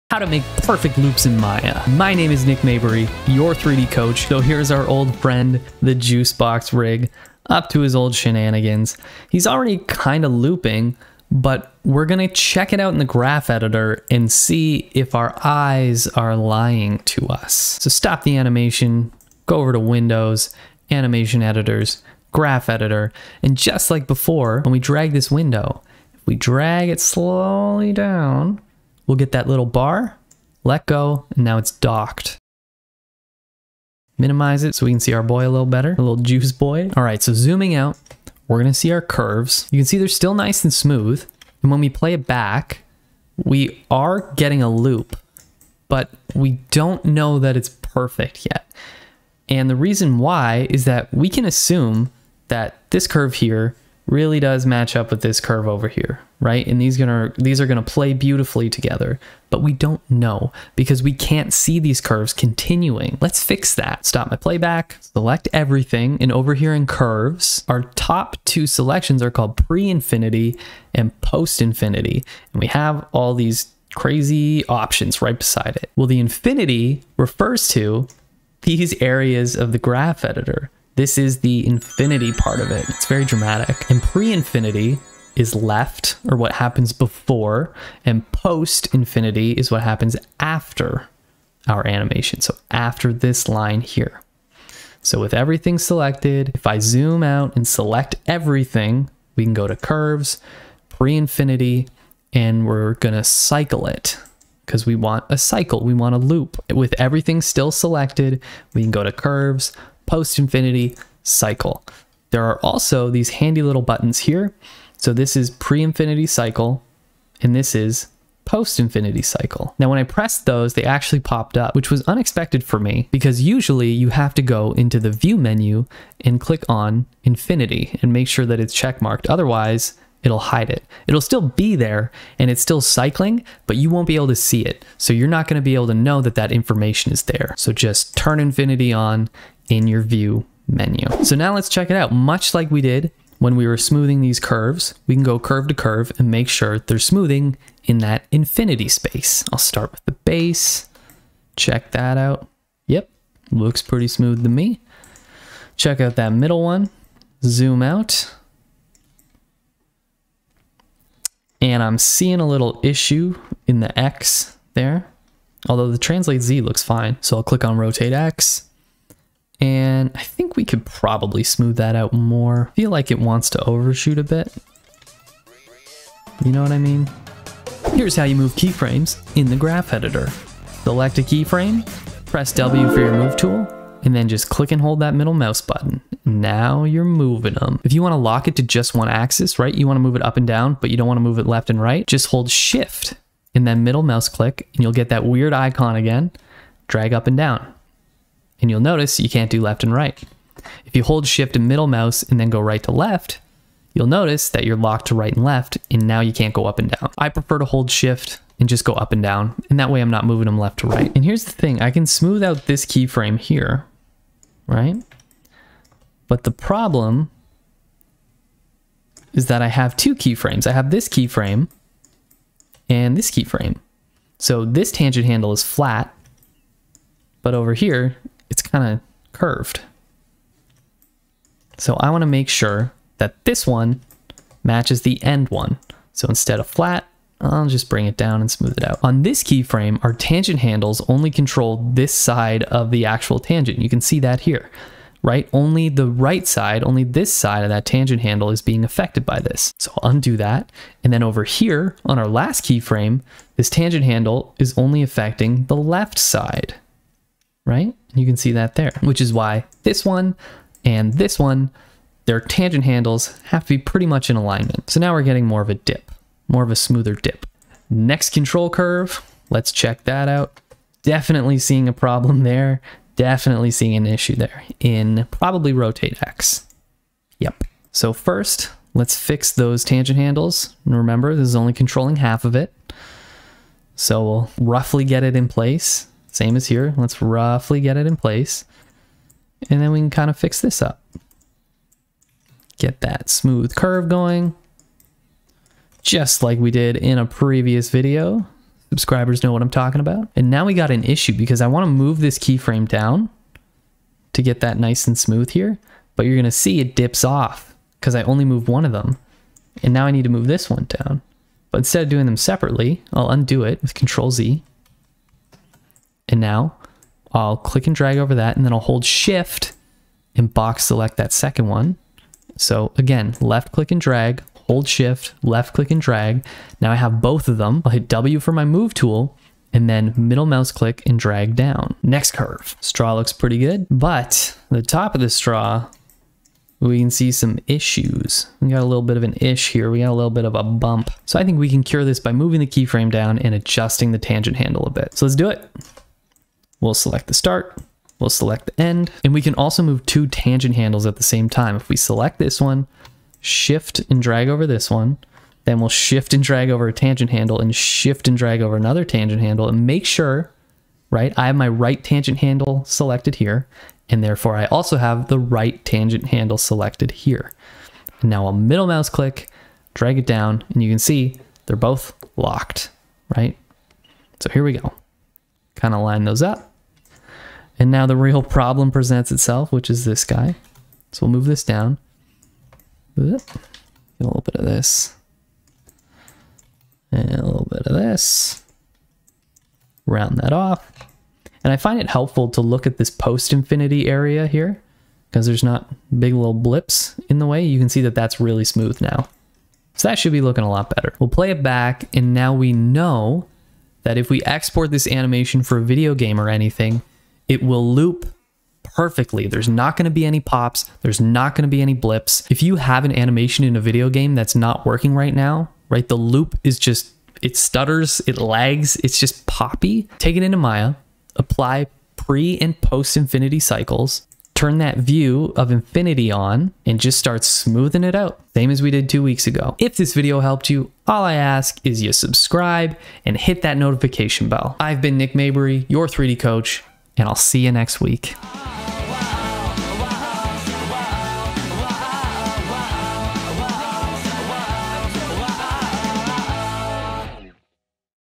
How to Make Perfect Loops (in Maya)
The secret of how to make perfect loops in Maya lies in the power of INFINITY! More specifically, the pre-infinity and post-infinity options in your graph editor.

So if you need perfect looping cycles for your game or show, endure this 7 minute tutorial. It's only 3.5 minutes on 2x speed ;)

Oh, and one last thing: My animation was 28 frames long, but frame 28 and frame 1 are identical. If you want a perfect loop, follow all the steps in the tutorial, but then move your timeline back 1 frame. Instead of 1f-28f, you'll be at 1f-27f. This way, the same pose (1f and 28f) aren't playing twice. If they did play twice, you'd have a 2-frame hold and your loop would absolutely NOT be perfect.

It would help me out immensely to be over 1k subscribers as it unlocks community posts.

0:00 Intro
0:18 Dock the Graph Editor
0:37 Why the loop isn't perfect
1:04 Pre/Post Infinity
2:21 Smoothing curves
2:57 How to Navigate the Graph Editor
3:31 Understanding Start/End Curves
4:27 Smoothing Start/End Curves
5:50 Manipulating multiple tangent handles
6:40 Playback

About this channel:
3D Coach is your community for digital creative hobbyists.